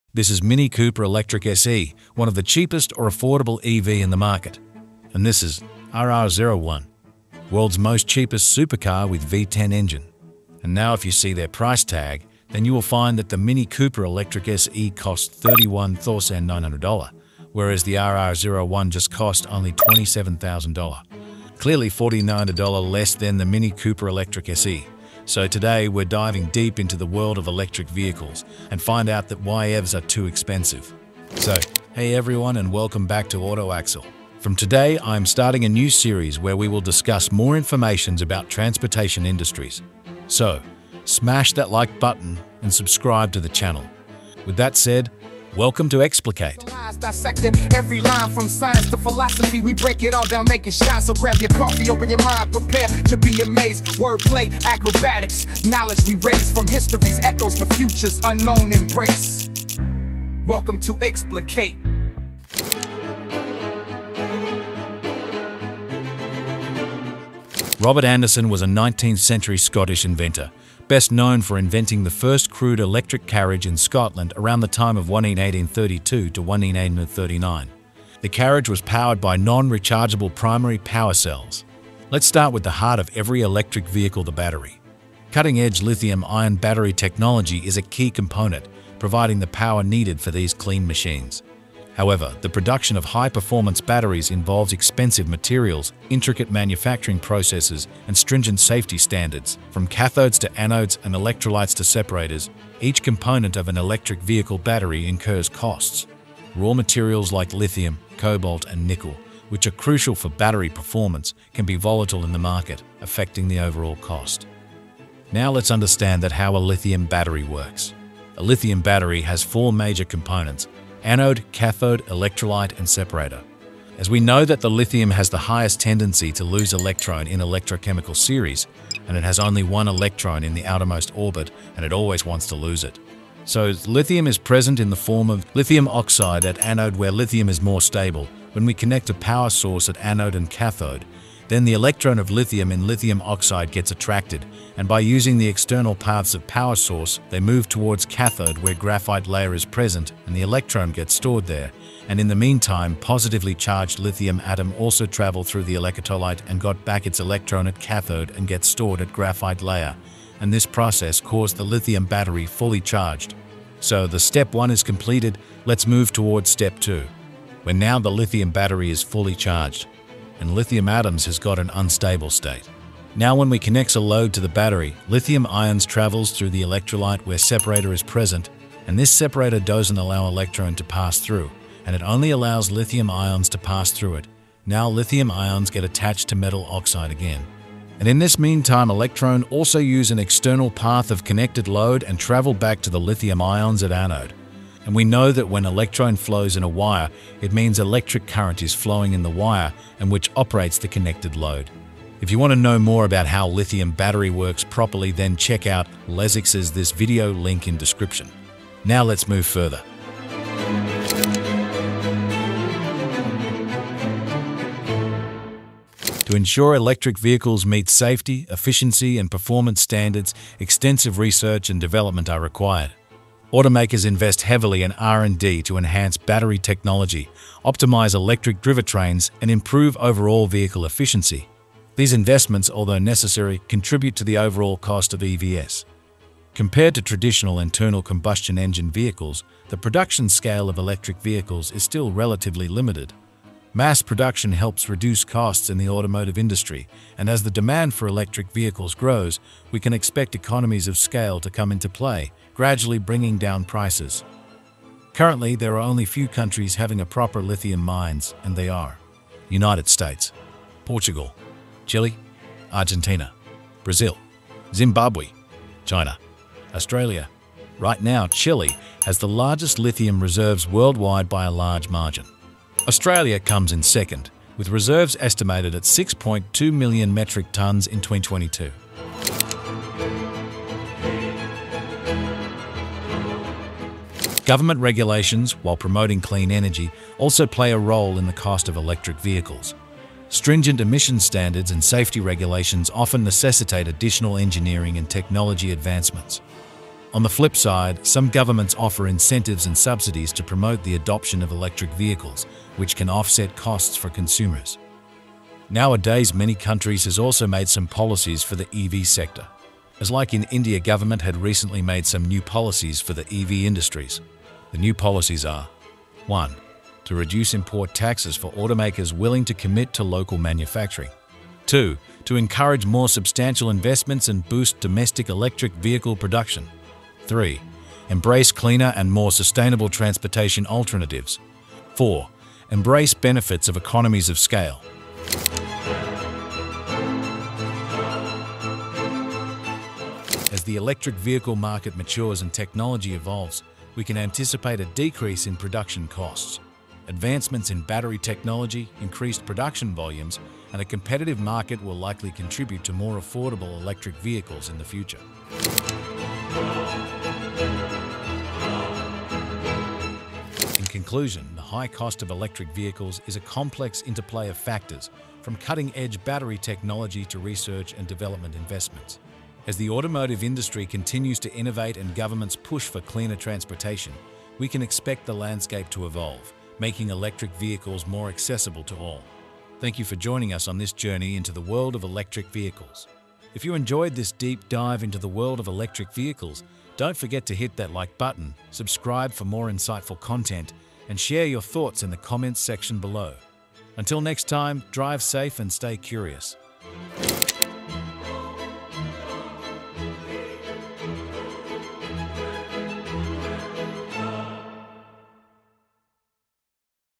The Untold Story of Why EVs Are Priced So High | AutoAxle | EXPLICATE EP01
In this episode of AutoAxle's EXPLICATE series, we uncover the untold story of why electric vehicles are priced higher than traditional cars. From battery costs to production challenges, find out the factors that contribute to the higher prices of EVs in today's market. If you're curious about the future of transportation and the rise of electric vehicles, this video is a must-watch!